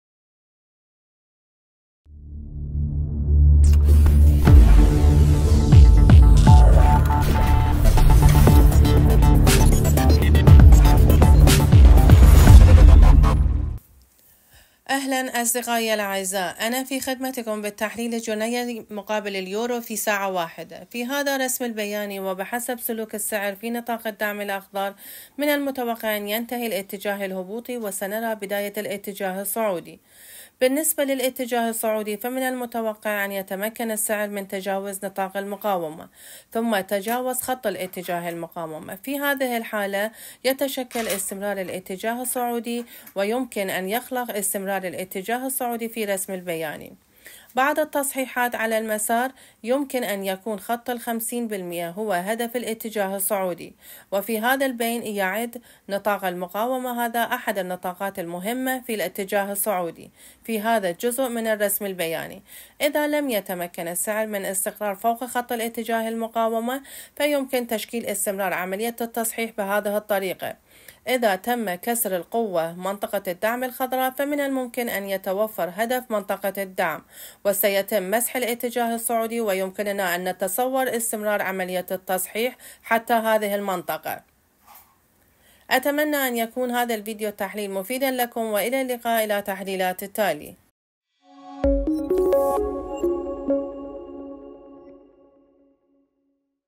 STPTRADING |تحليل اليورو مقابل الجنيه البريطاني على الإطار الزمني لمدة ساعة (3 سبتمبر)
نشارك الأخبار الأسبوعية وتحليل الأخبار على قناة يوتيوب. يمكنك استخدام هذه الأخبار في معاملاتك. يمكنك أيضًا الانضمام إلى الروابط أدناه واستخدام تدریب،و التحليل اليومي للفورکس